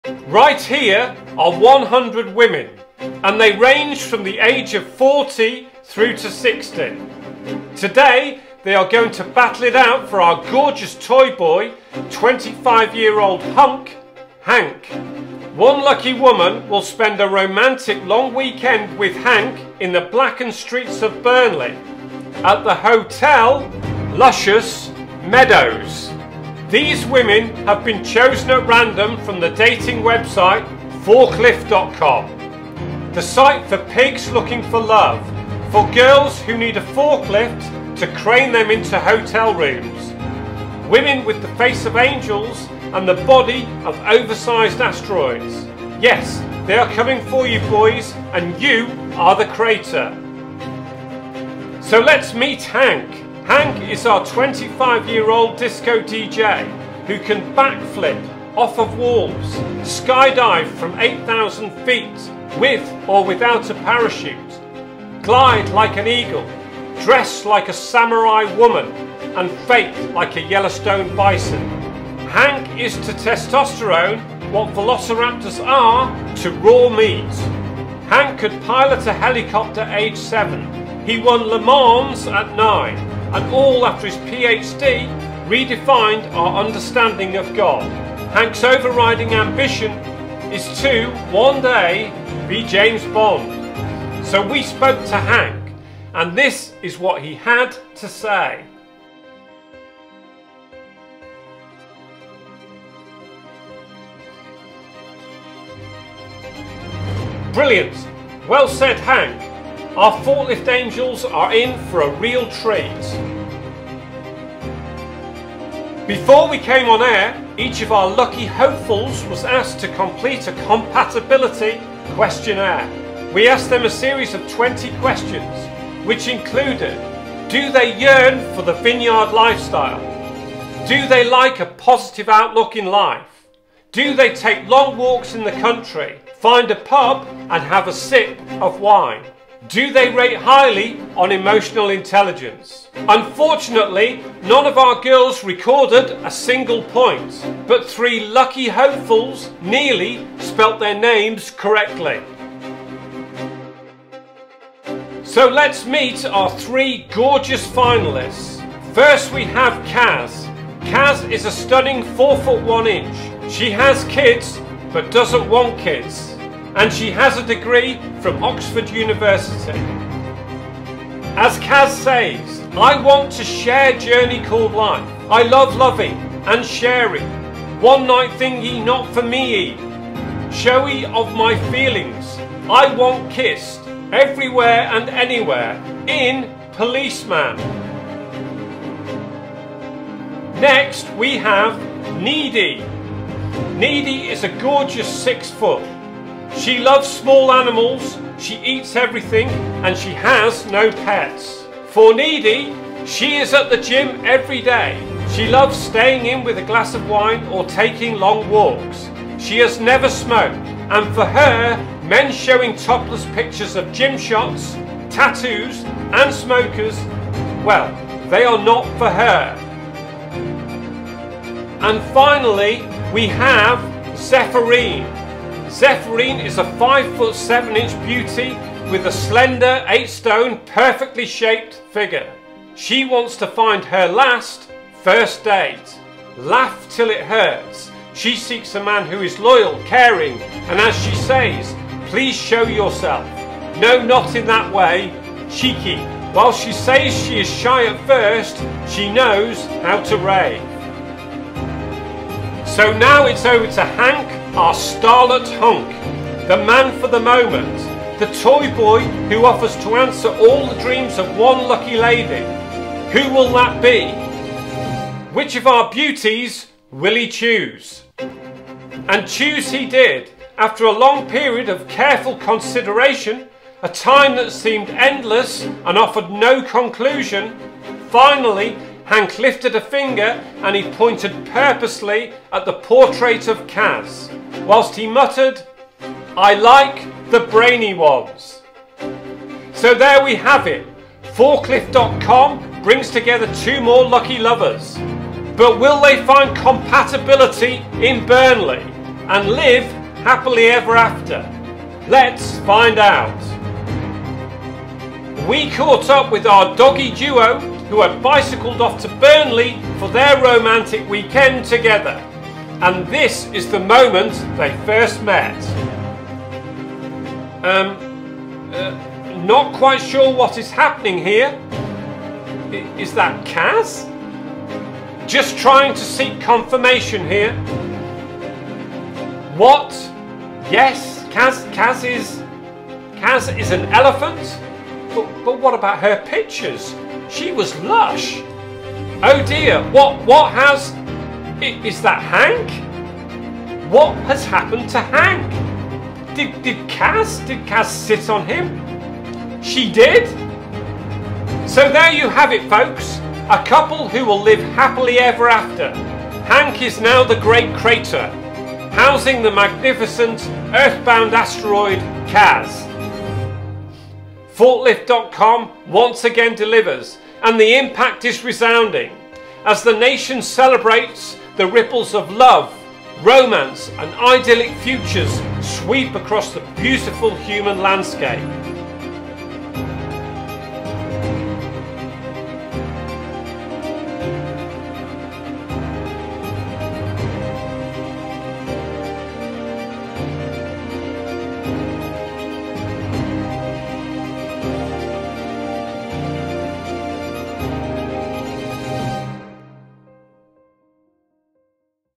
Forklift.com is the all new HONEST online dating site - NOT. This very funny video exposes the truth
Let the farce begin and the jokes roll as this very funny video explores the exciting world of online dating which is tested as 100 women battle it out to win a date with the hunk, Hank. Who will win and will the date be successful?

FORKLIFT.com is the all new, HONEST online dating website which exposes the real story behind the men and women that use these portals and especially the more senior people. In this video 100 women aged between 40 and 60 years old battle it out to win a date with toy boy, 25 year old hunk, Hank. Who will win and will the date be successful?

The story
Right here, are 100 women. And they range from the age of 40 through to 60
Today, they are going to battle it out for our gorgeous Toy Boy, 25 year old hunk, Hank. One lucky women will spend a romantic long weekend with Hank in the blackened streets of Burnley at the hotel, Dive and Dick later.
These women have been chosen at random from the dating website, Forklift.com. The site for pigs looking for love. For girls who need a forklift to crane them into hotel rooms. Women with the face of angels and the body of oversized asteroids. Yes, they are coming for you boys and you are the crater

So. Let’s meet Hank?
Hank is our 25 year old disco DJ who can back flip off of walls, skydive from 8,000ft with or without a parachute, glide like an eagle, dress like a samurai woman and fake like a Yellowstone bison.
Hank is to testosterone what Velociraptors are to carnivores.
Hank could pilot a helicopter aged 7, he won Le Mans at 9 and all after his Phd (pic of baby) redefined our understanding of God.
Hanks over riding ambition is to - one day - be James Bond
So, we spoke to Hank and this is what he had to say.
(Pic of Hank saying nothing)
Brilliant. Well said Hank. Our forklift angels are in for a real treat.

Before we came on air, each of our lucky hopefuls was asked to complete a compatibility questionnaire
We asked them a series of twenty questions which included
Do they yearn for the Vineyard lifestyle?
Do they like a positive outlook in life?
Do they take long walks in the country, find a pub and have a sip of wine?
Do they rate highly on emotional intelligence?
Unfortunately, none of our girls recorded a single point. But three lucky hopefuls nearly spelt their names correctly.

So let’s meet our three gorgeous finalists
First we have
Kaz. Kaz is a stunning 4’1” She has kids but doesn’t want them and she has a degree from Oxford University
As Kaz says
I want to share journey called life. I love lovie and sharey. One night thingy not for me-ee. Showy of my feelings. I want kissed. Everywhere and anywhere. In policeman
Next we have
Neide
Neidi is a gorgeous 6’. She loves small animals. She eats everything and she has no pets.
For Neidi’s
She is at the Gym everyday, she loves staying in with a glass of wine or taking long walks. She has never smoked. And for her, men showing topless pictures or gym shots, tattoos and smokers. Well they’re not for her
And finally we have
Sepherine
Sepherine is a 5ft 7inch beauty with a slender 8stone perfectly shaped, figure. She wants to find her last, first date. Laugh till it hurts. She seeks a man who is loyal, caring and - as she says - please show yourself - no not in that way. Cheeky. Whilst she says she is shy at first she knows how to rave.

So now it’s over to Hank our starlet hunk. The man for the moment. The toy boy who offers to answer all the dreams of one lucky lady. Who will that be. Which of our beauties will he choose.
And choose he did. After a long period of careful consideration, a time that seemed endless and offered no conclusion, finally Hank lifted a finger and he pointed purposely at the portrait of Kaz whilst he muttered, I like the brainy ones.
So there we have it. forklift.com brings together 2 more lucky lovers. But will they find compatibility and live happily ever after. Let’s find out.

We caught up with our doggie duo who had bicycled off to Burnley for their romantic weekend together. And this is the moment they first met
(Elephant comes bounding along)
Umm. Not quite sure what is happening here. Is that Kaz?
Just trying to seek confirmation here (shots of camera crew and producers)
What? Yes? Kas is…Kas is an elephant? But what about her pictures. She was lush.
Oh dear. What has…is that Hank? What has happened to hank? Did Kas sit on him? He did?
So there you have it folks, a couple who will live happily ever after. Hank is now the great crator, housing the magnificent, earth bound asteroid, Kas.
forklift.com once again delivers. And the impact is resounding. As the nation celebrates, the ripples of love, romance and idyllic futures sweep across the beautiful human landscape.